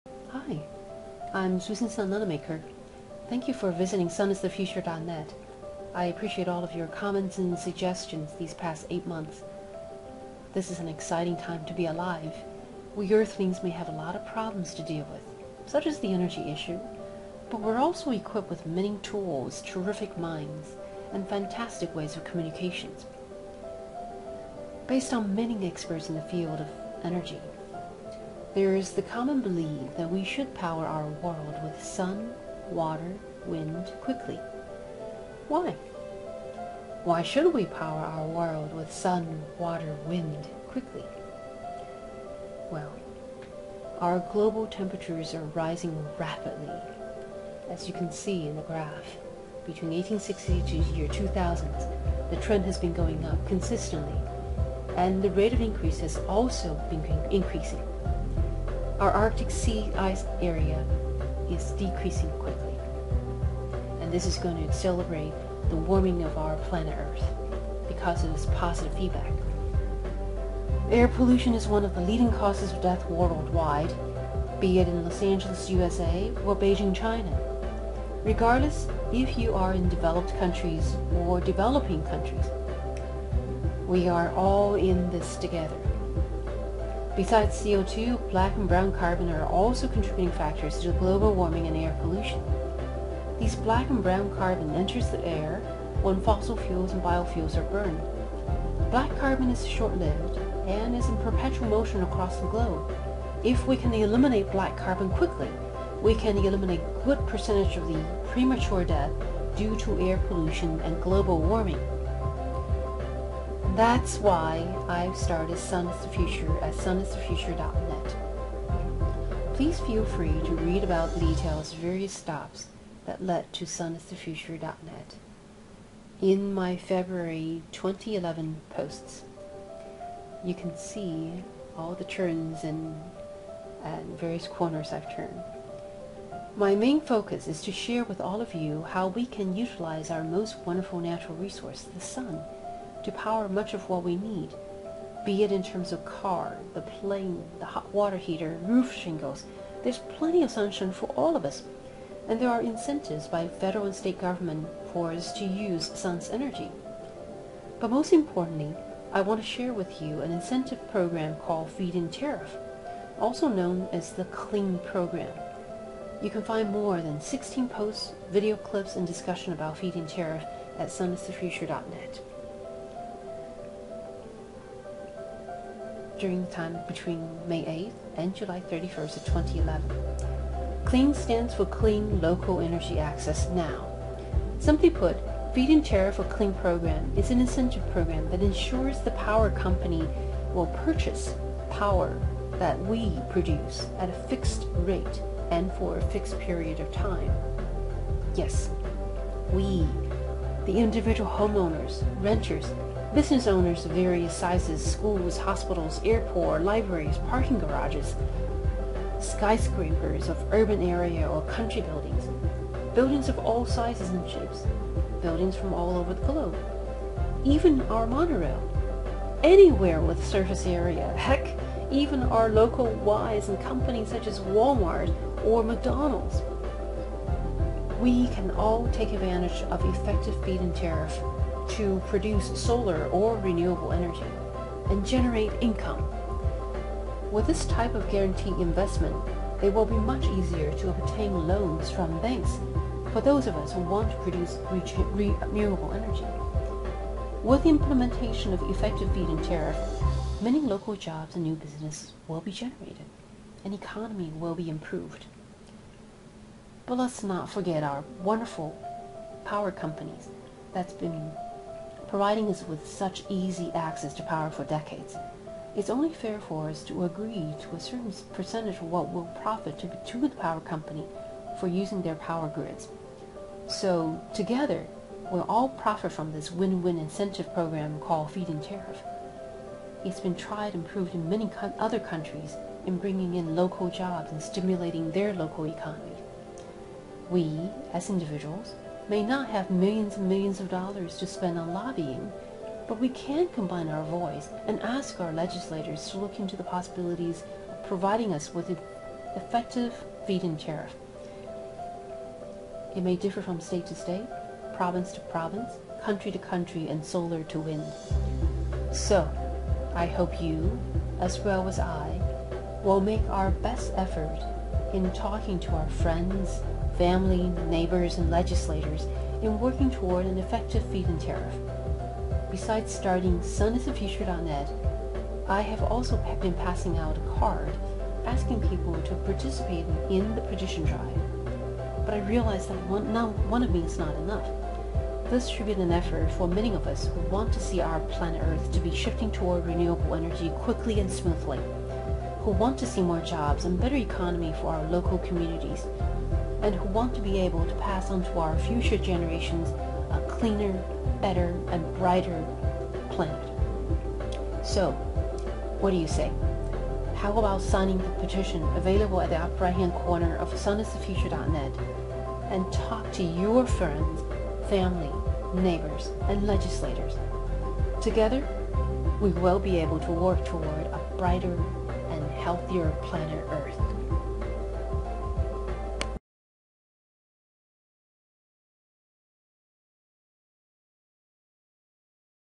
Intro to petition for effective feed in tariff at sunisthefuture.net
Please participate in the grass root movement for effective Feed-In-Tariff/CLEAN program , show your support for renewable energy by signing this petition available and talk to your family, friends, neighbors, and legislators about implementing effective Feed-In-Tariff/CLEAN program.  The effective Feed-In-Tariff will help to create local jobs, cleaner environment, and better local economy.  Let's join our effort together in moving toward the renewable energy world, for a brighter and healthier future.
More discussions and videos for Feed-In-Tariff available (May 8-July 31, 2011 posts)
sunisthefuture-Susan Sun Nunamaker
Let's work together in transitioning our planet earth toward the renewable/solar energy age. on solar energy.